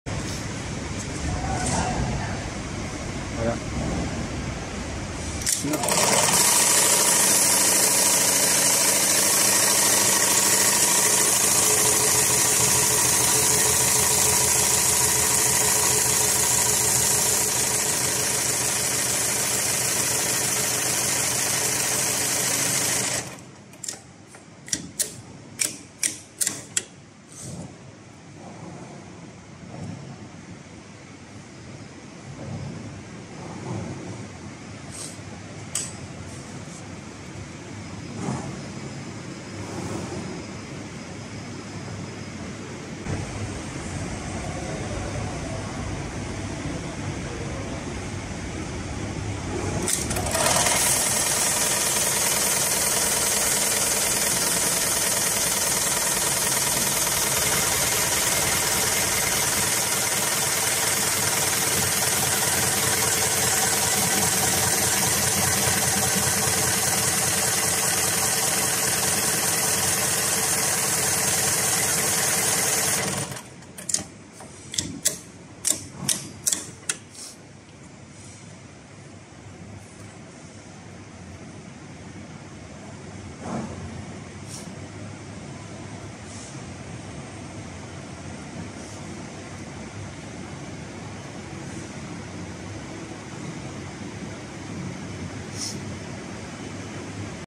High speed climbing rope sewing machine,safety rope sewing machine
The NEW HighTex 7273PLK/R extra heavy duty, automatic & programmable bar-tacking sewing machine can make a revolution in your sewing production of climbing rope and safety rope.
High sewing quality even at high-speed sewing (max. 800 RPM)
Equip with automatic thread trimmer (hot cutter).
Perfect for climbing rope manufacturers and safety rope manufacturer who look for highest efficiency sewing machine.
Test 10mm safety rope at speed of 800 RPM!
Test 14mm safety rope at speed of 600 RPM!!